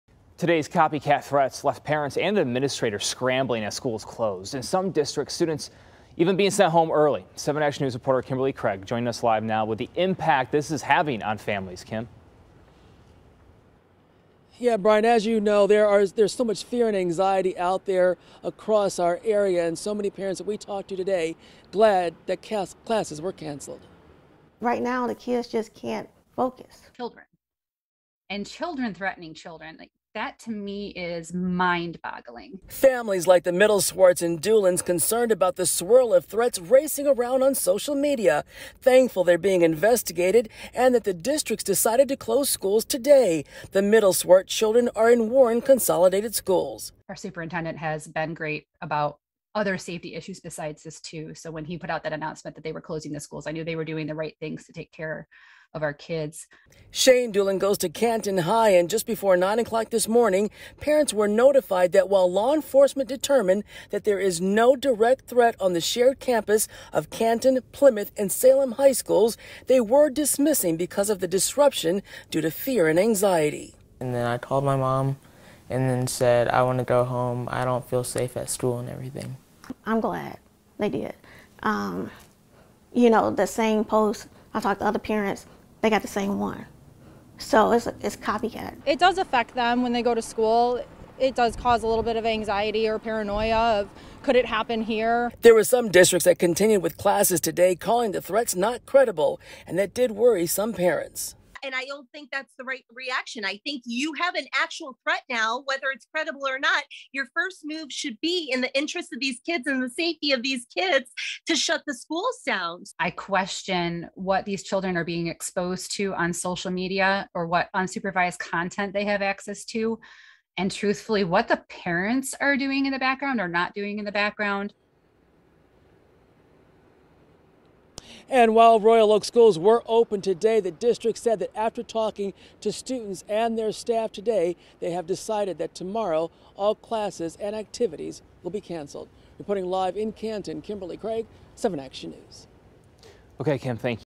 Parents worry about affect of copycat threats
Parents of students at schools who closed due to copycat threats are worried about the effects those are having on their children.